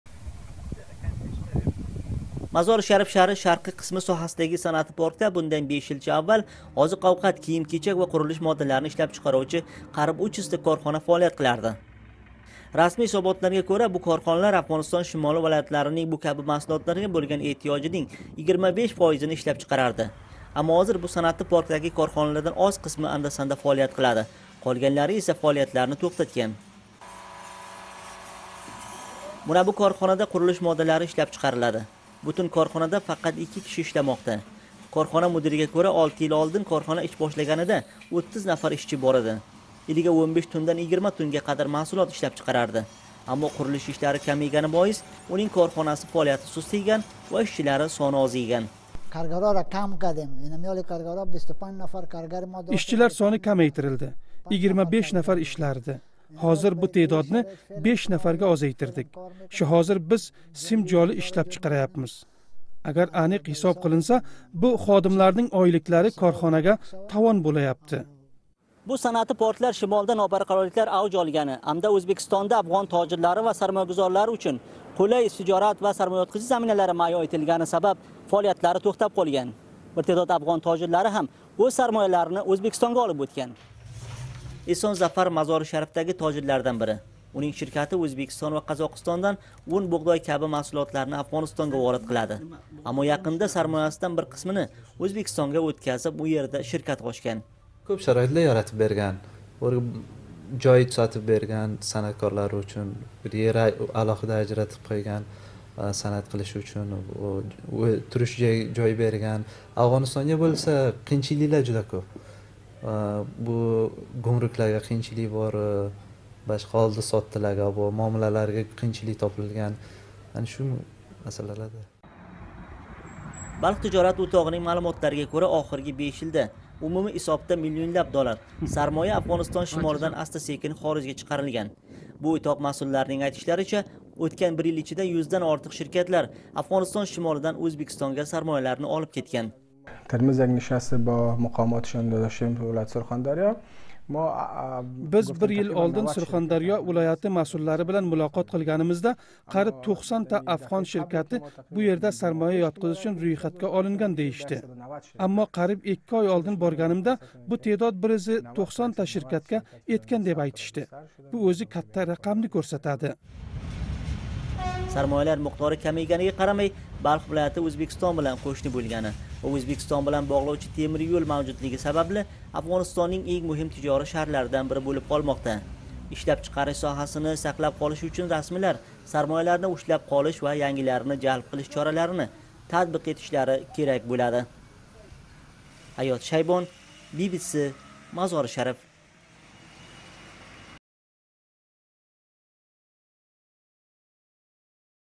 Афғон бизнеслари Ўзбекистонга кўчиб ўтмоқдами? - BBC Uzbek
Афғонистоннинг Мозори Шариф шаҳридаги саноат паркида 5 йил илгари қарийб 300 ишлаб чиқариш корхонаси фаолият юритарди.

У ерда озиқ-овқат, кийим-кечак ва қурилиш моллари ишлаб чиқарилган.

Расмий маълумотларга кўра, бу корхоналар Афғонистон шимолий вилоятларининг бундай маҳсулотларга бўлган эҳтиёжининг 25 фоизини қондирган.

Бугунги кунда саноат паркида бармоқ билан санарли корхоналар қолган.

Аксар Афғон тадбиркор ва тужжорлари Ўзбекистоннинг Сурхондарё вилоятида бизнес юритиш учун қулай шароит яратилгани, Афғонистондаги хавфсизлик вазиятини ҳам ҳисобга олиб бизнесларини Ўзбекистонга кўчирганларини айтадилар.

Би-би-си Ўзбек хизмати мухбири Ҳаёт Шайбон тайёрлаган видео лавҳани томоша қилинг.